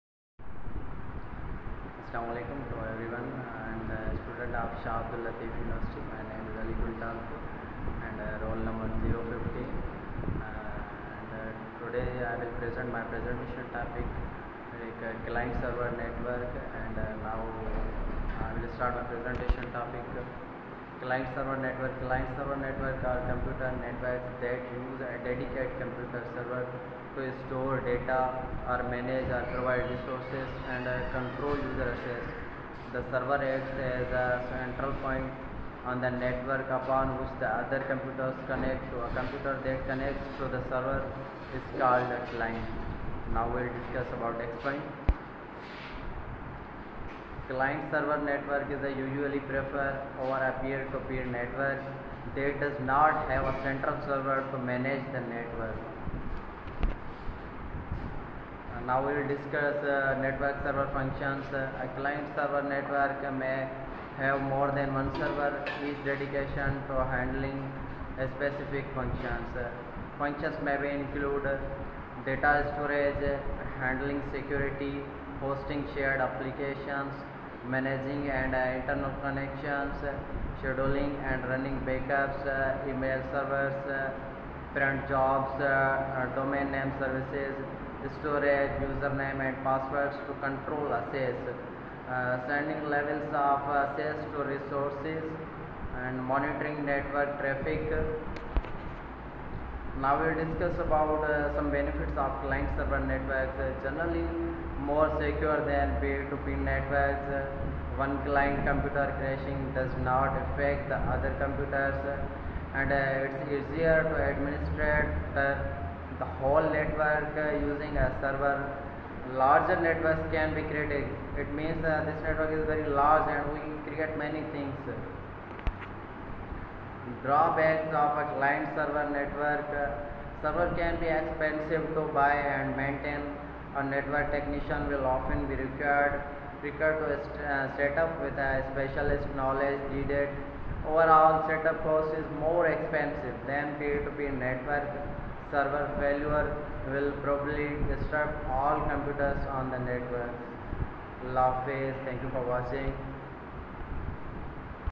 Lecture on Client-Server Network Ali Gul Talpur Shah Abdul Latif University
Client-Server Network